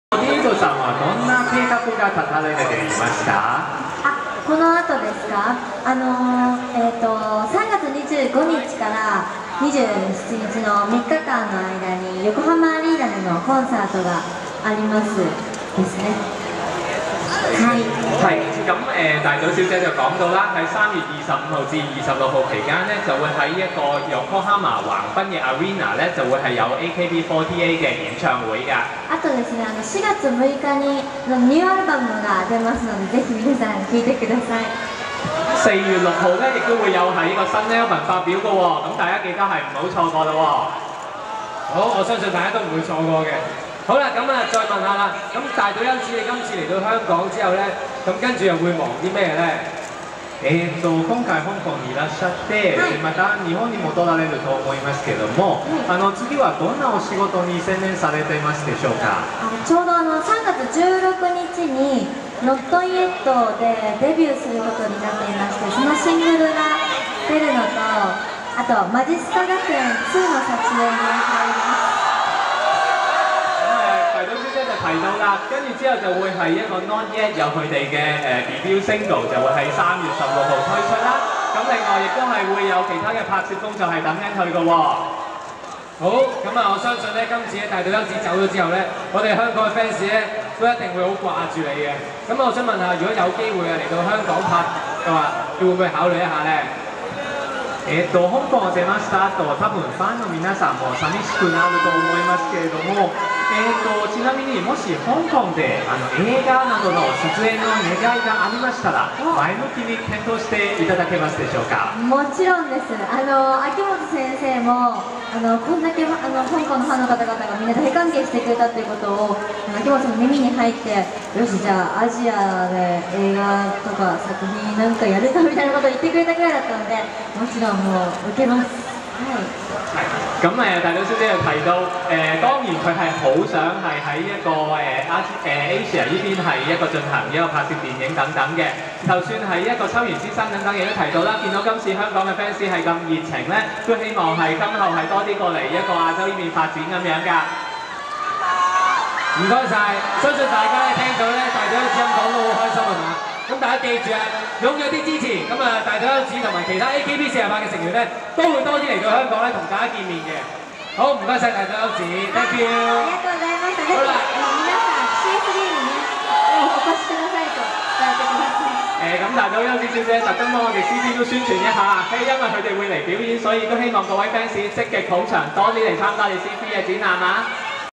【大島優子】透露AKB48未來動向- C3日本動玩博覽2011發佈會@西九龍中心
3711 - 日本動漫玩具展覽的真正元祖級盛事，每年盛夏於東京舉行的「C3 x HOBBY展覽」，吸引各地動漫迷入場欣賞；令觀眾目不暇給的最新動玩商品及精彩表演，均成為最高動漫話題！

為隆重其事，大會於27/2（星期日）假深水埗西九龍中心舉行記者發佈會。將出席「C3日本動玩博覽2011」，並舉辦「第2回AKB48才藝表演大賽」的超人氣女子組合AKB48，更派出了主將大島優子支持今次活動，全力宣傳這次動漫界的盛事﹗

在大會代表出原隆史先生上台致辭過後，本地Cosplay少女跳舞組合「V-Project」便以亮麗裝扮上台熱舞。緊接下來AKB48 2010年總選第1位：大島優子在一片歡呼聲中壓軸登場。大島優子與司儀閒談過後便與現場觀眾玩問答遊戲並跟得獎觀眾拍攝合照，令場面一度沸騰。最後眾嘉賓上台拍攝過大合照後，活動亦在一片熱鬧中圓滿結束。